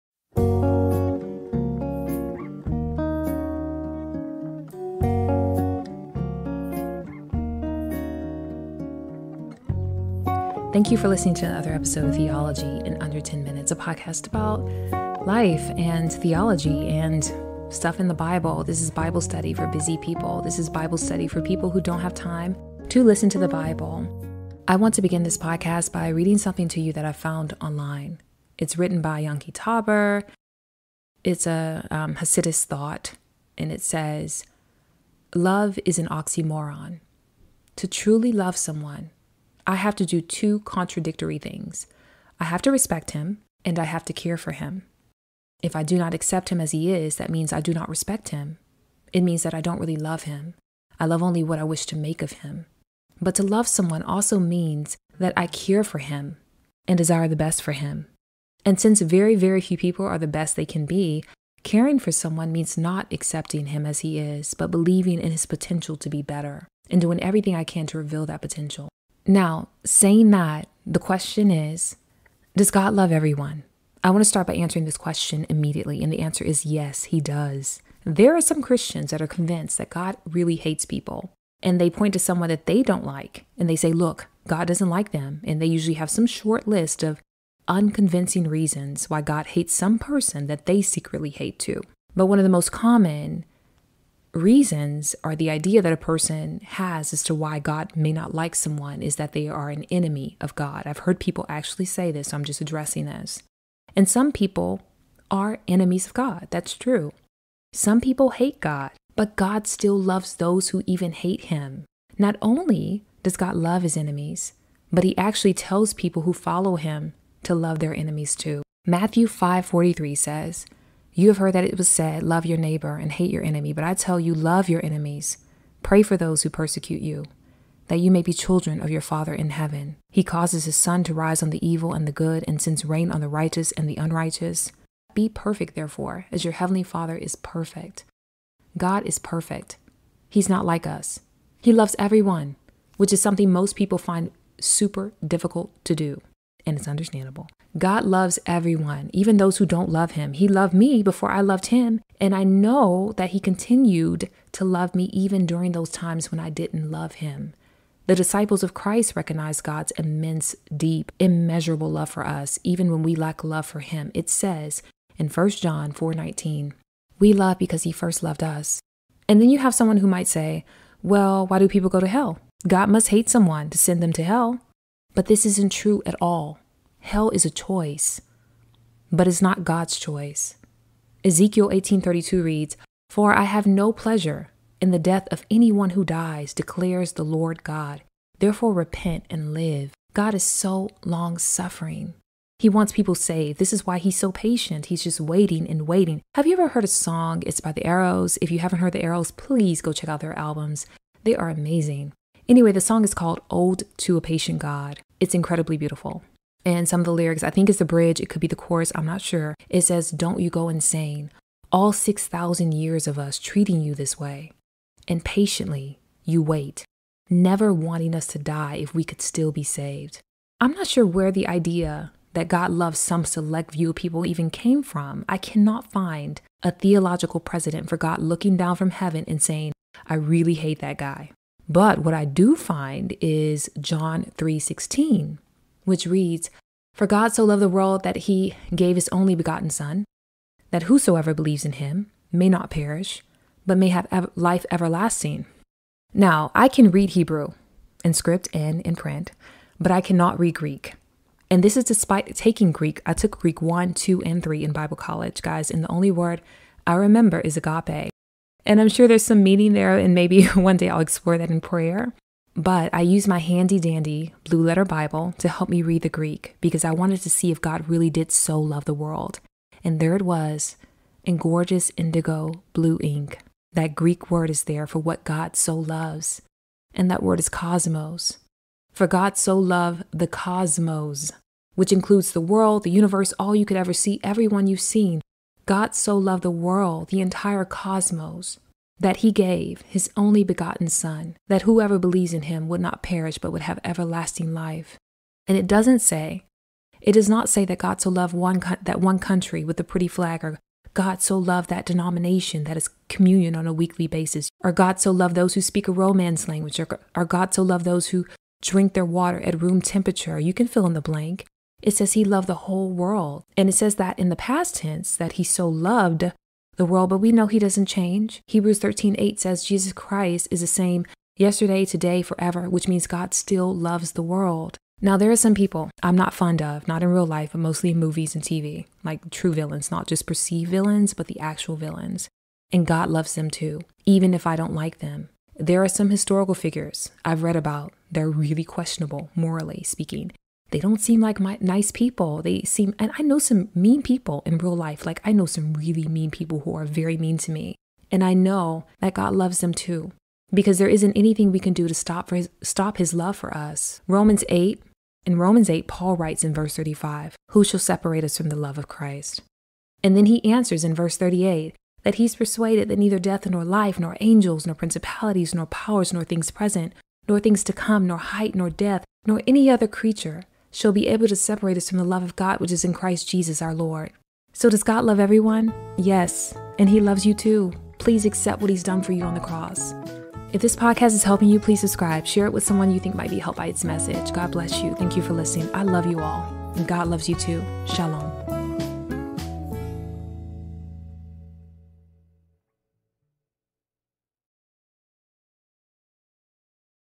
Does God Really Love Everyone?
Does God Really Love Everyone? In this episode, we explore if God loves everyone or just some people.